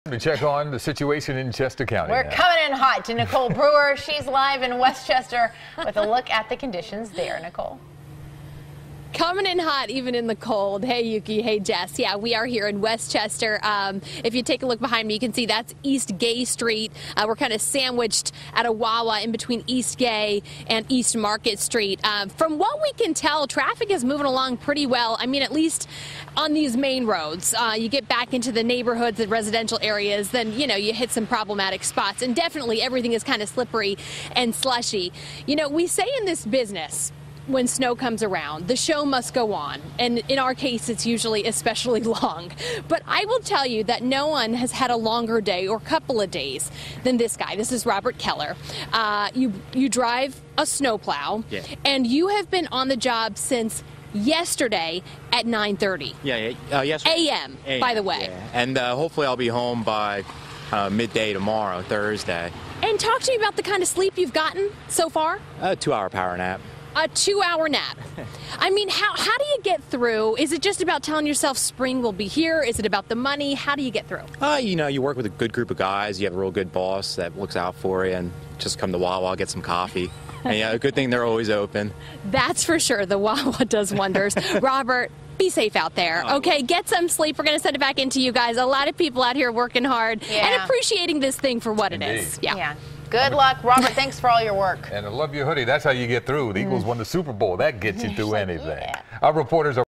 Snow Plow Driver Clearing Roads In West Chester
CBS3's Nicole Brewer reports.